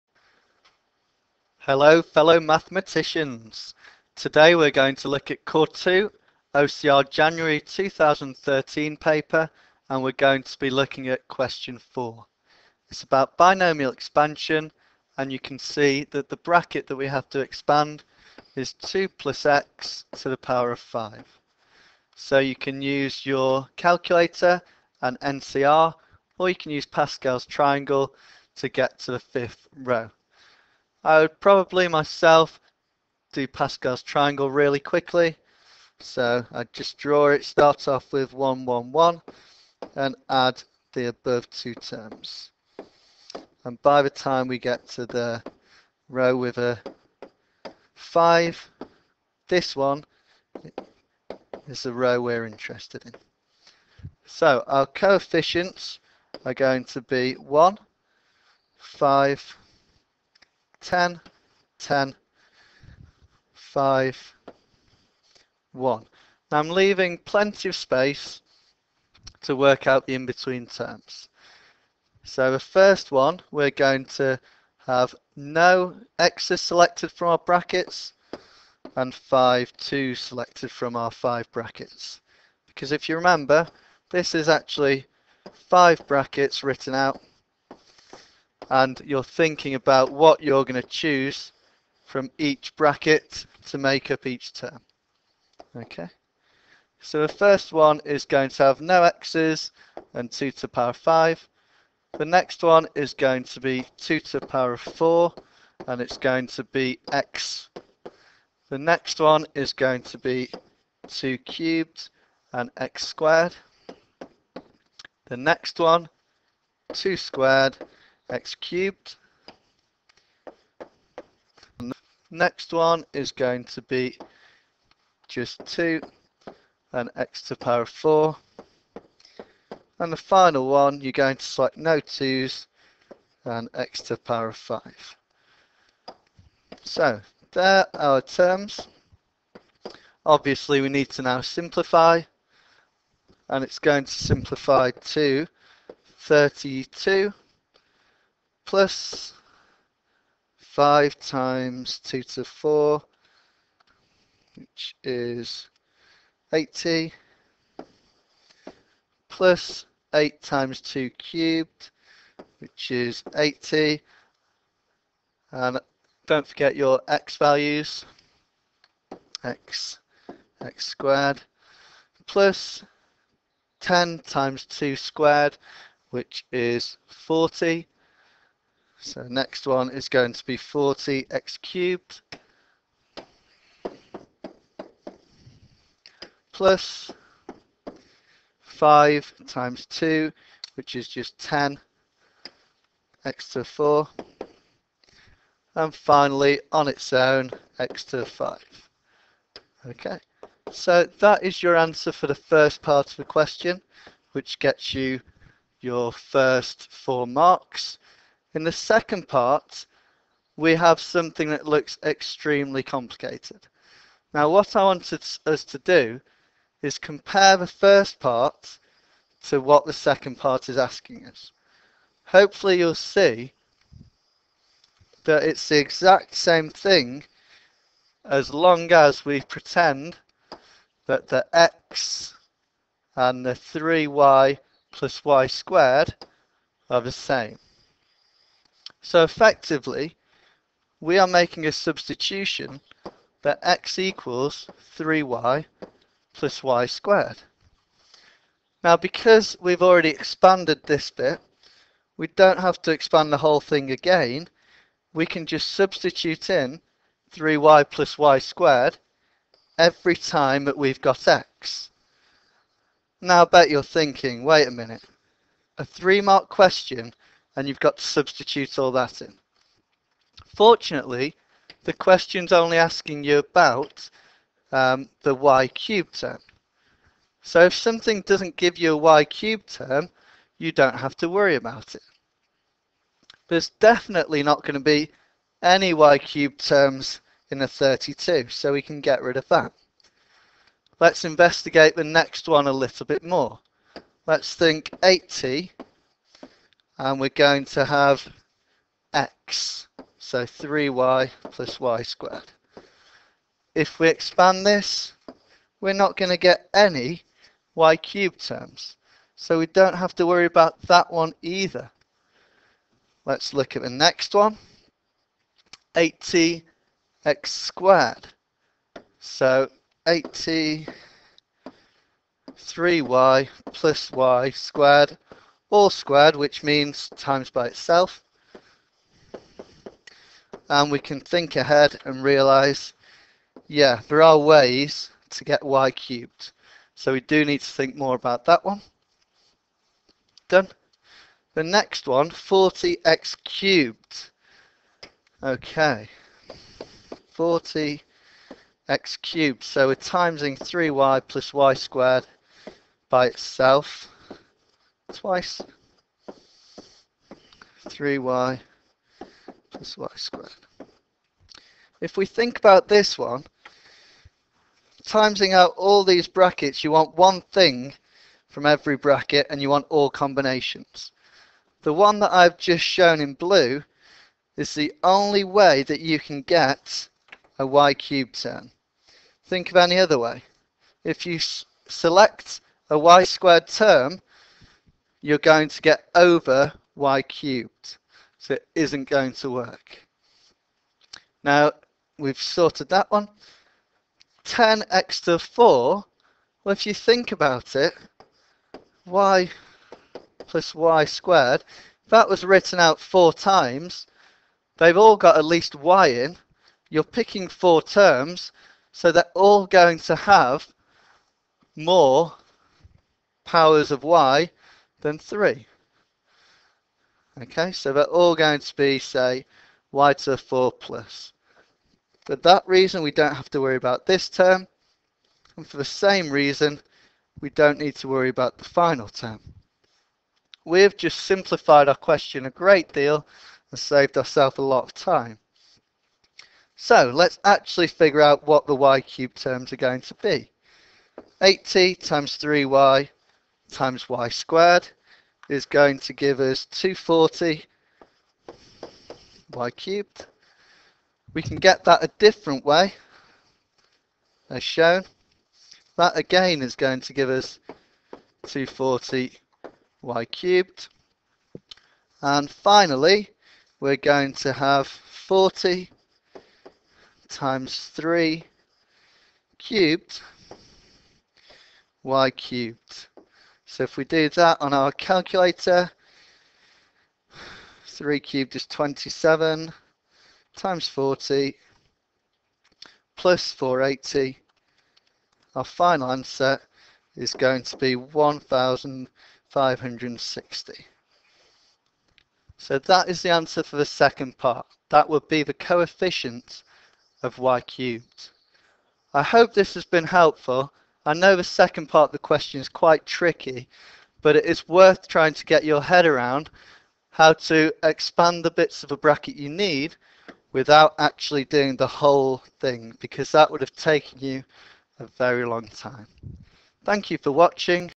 A-level Maths C2 Core 2 OCR January 2013 q4 - Binomial Theorem
Tutorial showing how to use the binomial theorem to expand brackets and how to use substitution within this.